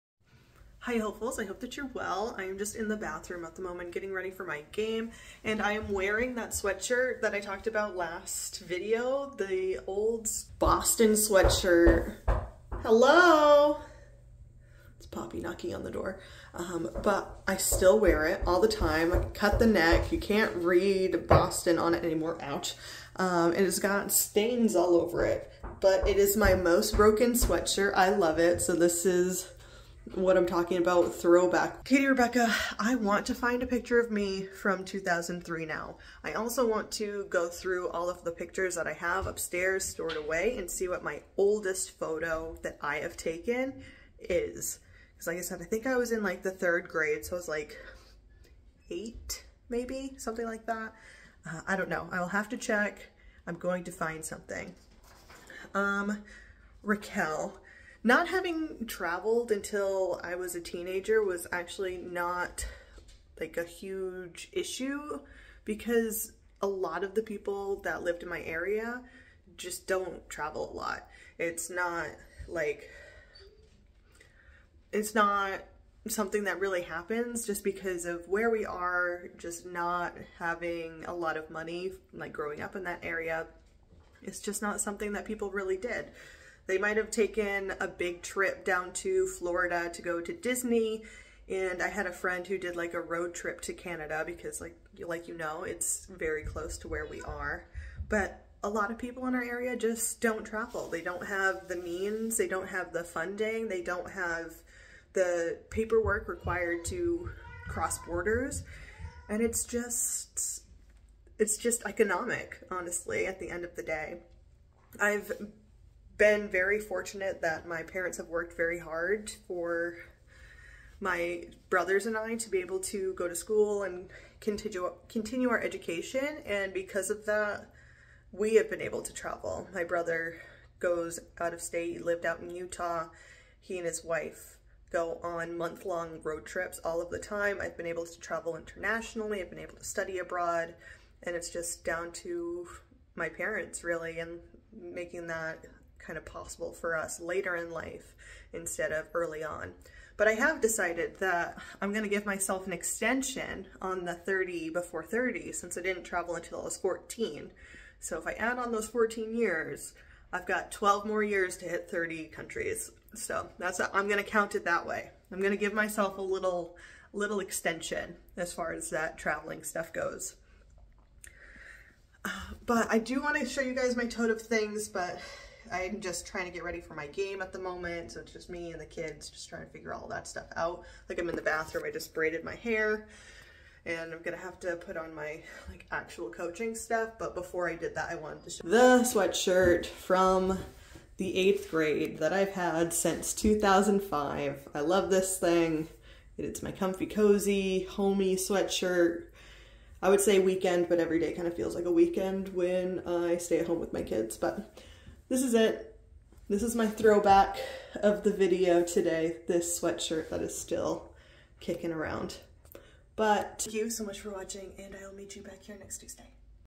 13 Year Old Hoodie || Samantha Kary
To follow up on my photos from my first ever trip out of state I present the hoodie I bought 13 years ago! I love a broken in hoodie and I can't imagine a time where I would ever think to throw out this relic!

↓LINKS BELOW↓

✰RAQUEL🇳🇱
Twitter, Pinterest and Instagram: @mynameiskellyYT